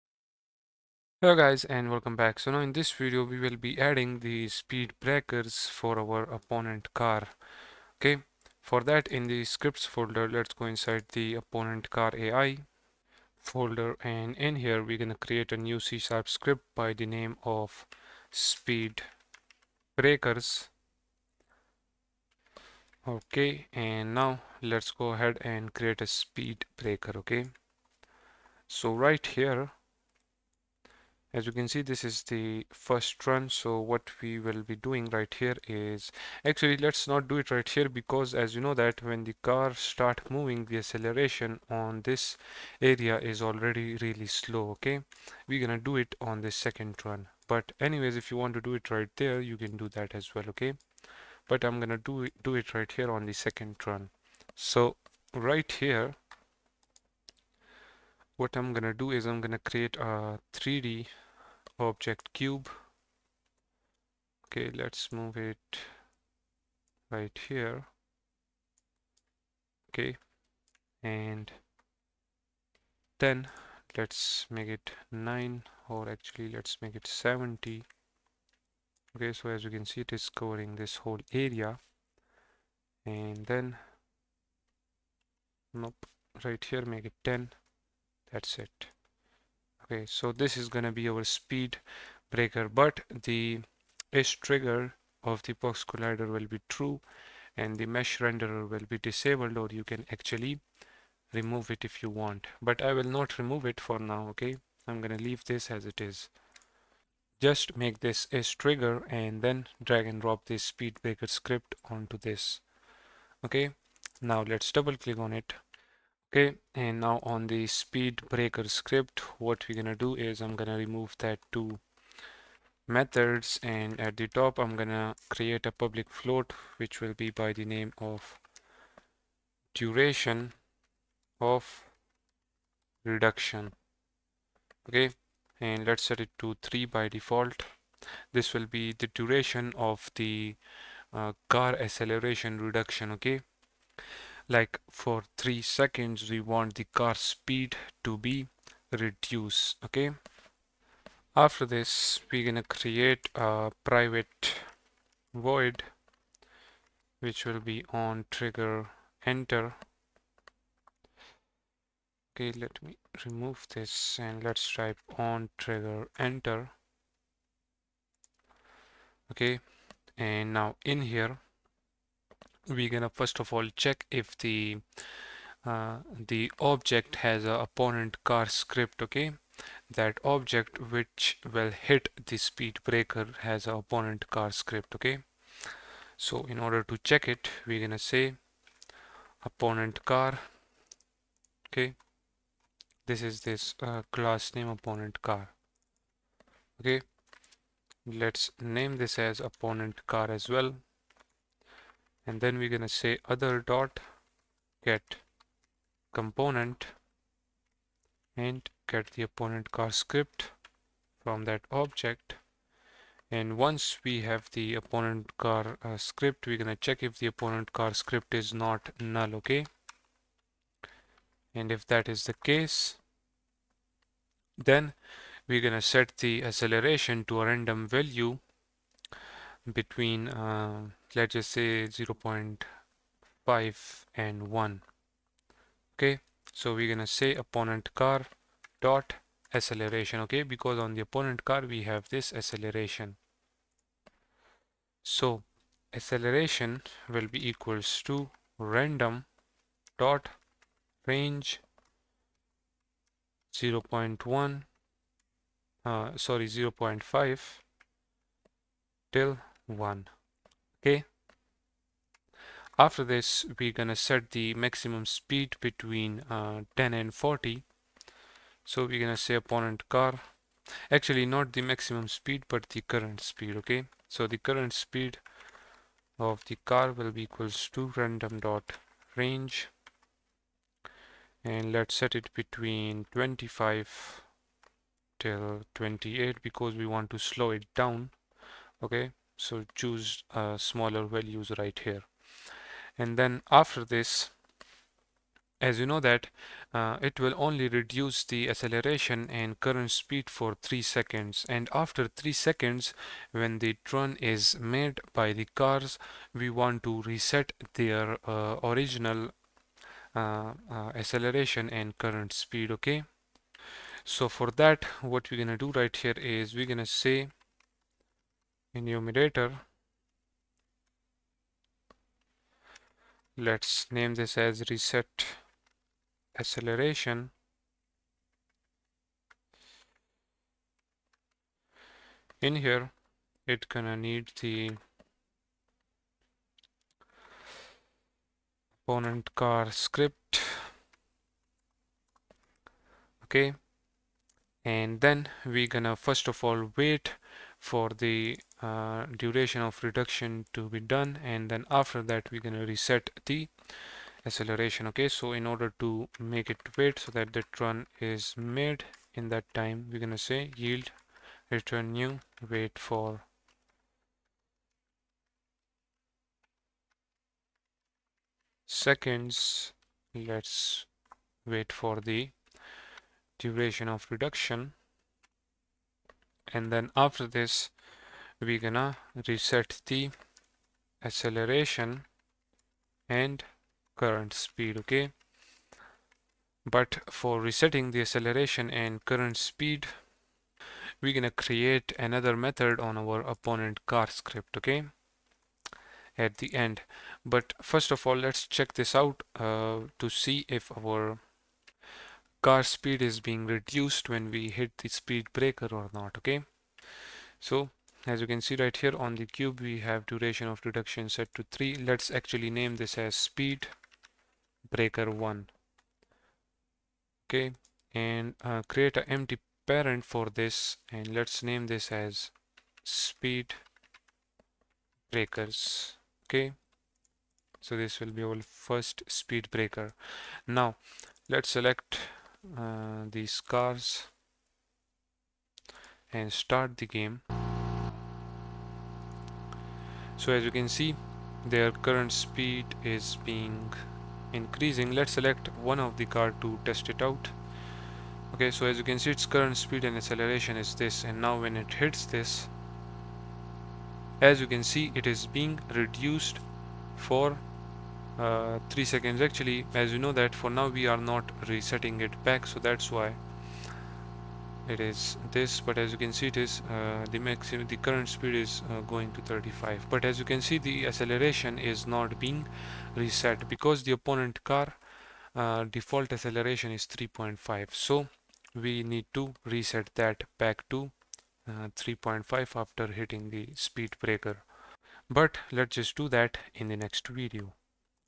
Unity Speed Breaker for Opponent Car | Mobile Game Development Full Course | Car Race Game
This is tutorial# 22. In this course you will learn and build unity c# ios and android mobile car racing game using unity game engine step by step.